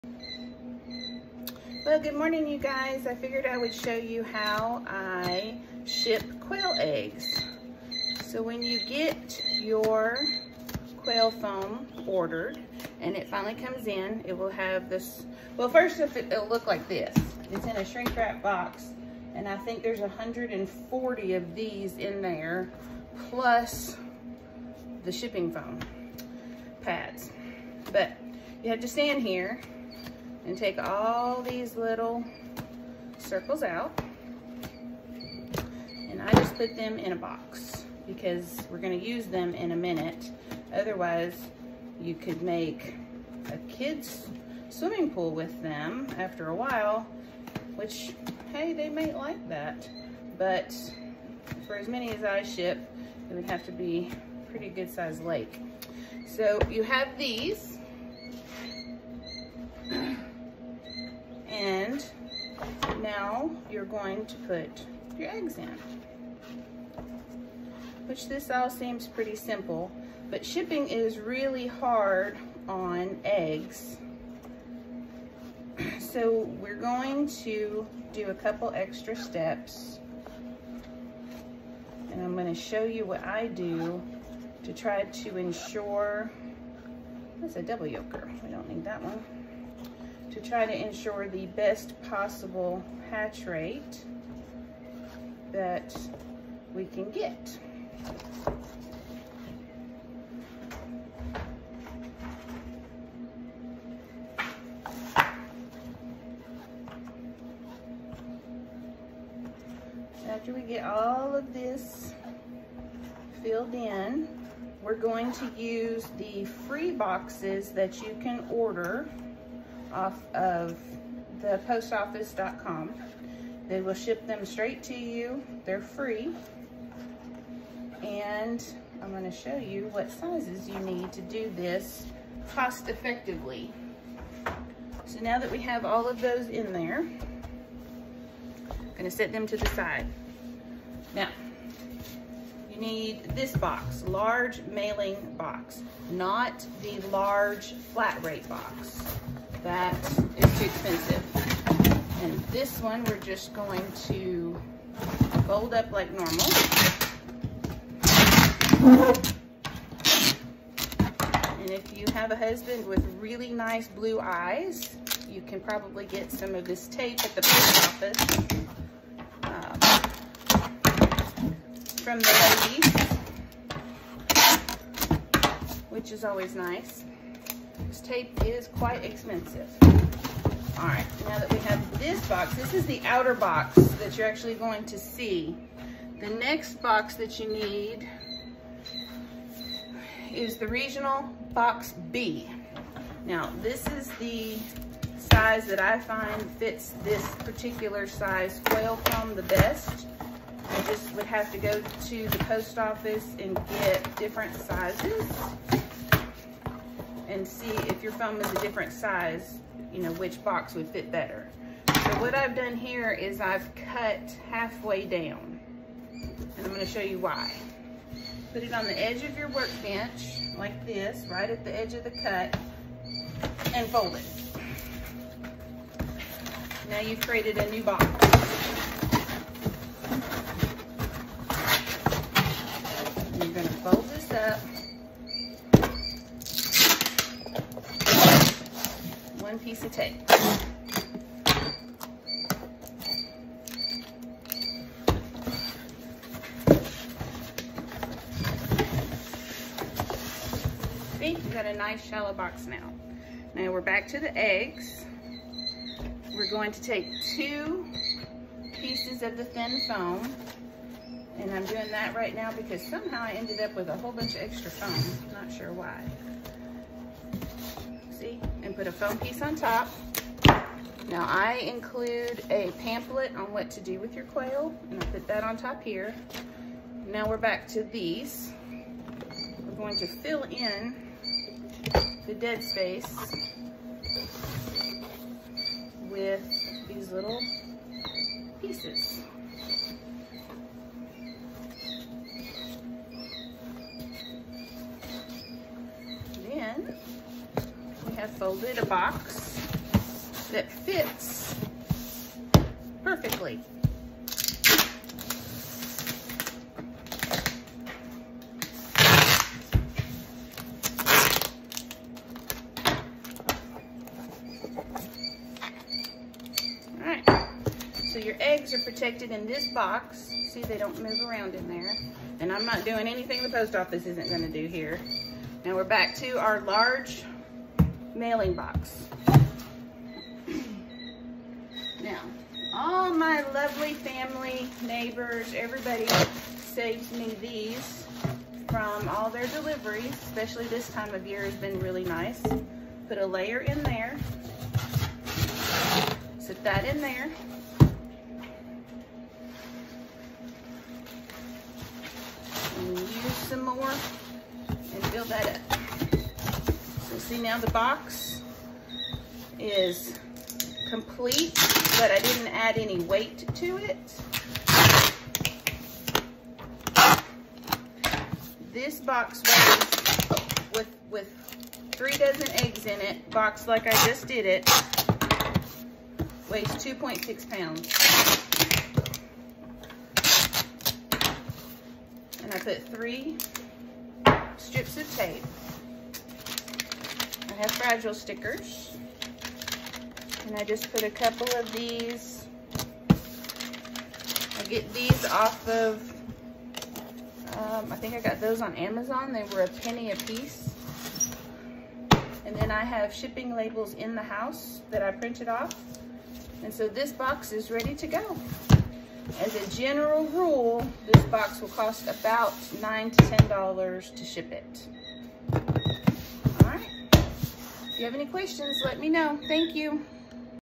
Quail egg shipping
I will show you how I box and ship quail eggs to insure the best outcome for hatching.  I use free usps boxes and foam shippers.